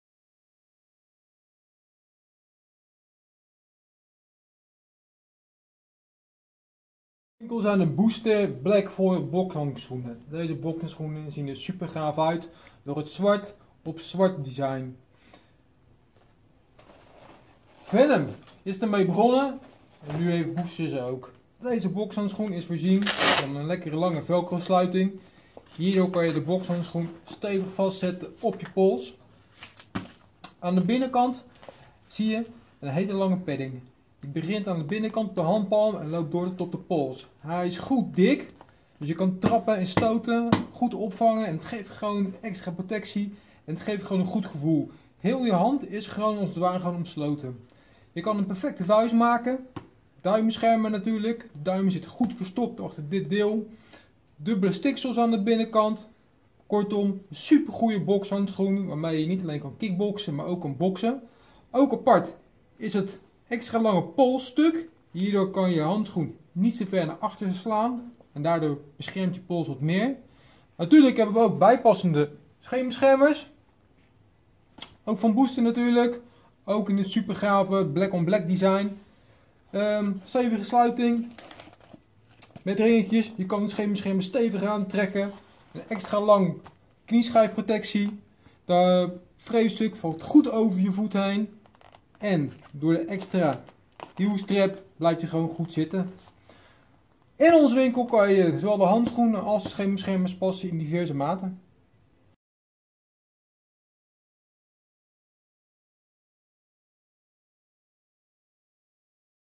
Bokshandschoenen Review: Booster Black Foil
Booster Black Foil Bokshandschoenen zijn gemaakt van een topkwaliteit leer. De Booster boxhandschoenen staan bekend om de goede vulling, de stevige klittenbandsluiting en goede duurzaamheid! In onze winkel hebben we diverse modellen Booster bokshandschoenen voor mannen en vrouwen. Alle gangbare gewichten zoals 10 oz, 12 oz, 14 oz en 16 oz hebben we in onze winkel. Bestel je liever online? Vandaag bestelt voor 16.00 is morgen in huis!